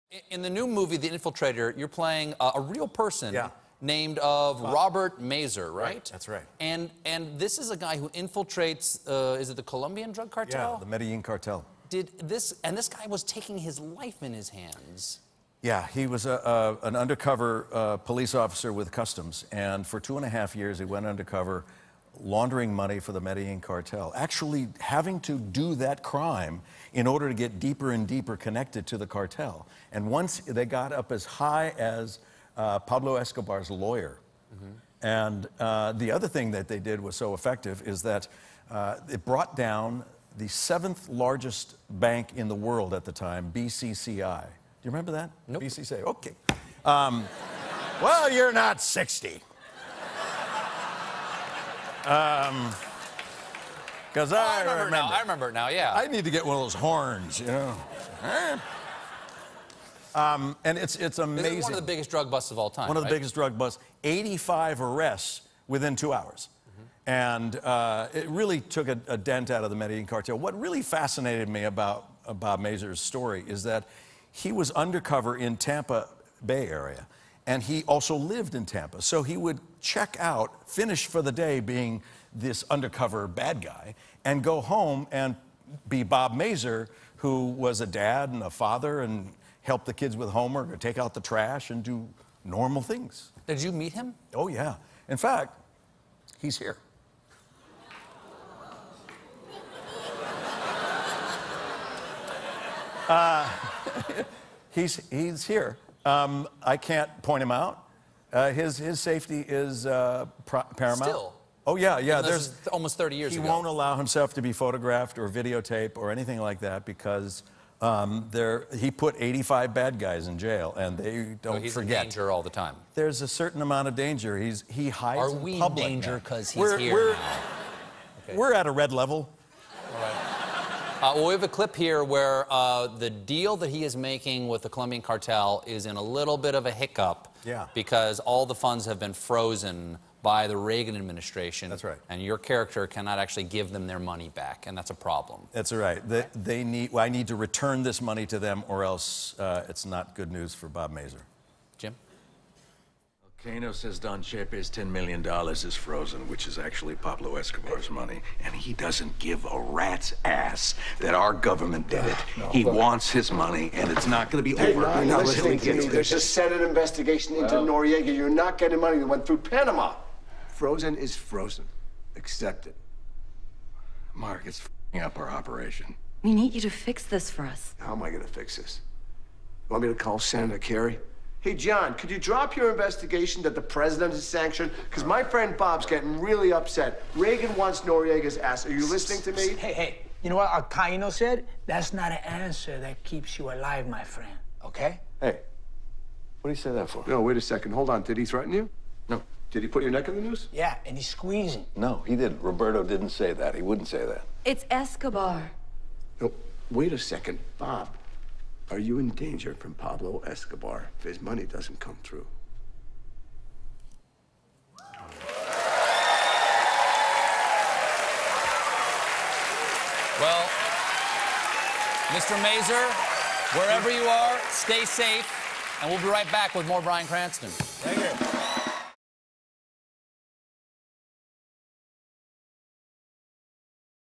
Bryan Cranston's Character Is In Constant Danger In "The Infiltrator"
The "Breaking Bad" star reveals the details of his new movie role, which puts him in greater danger than any of his previous films.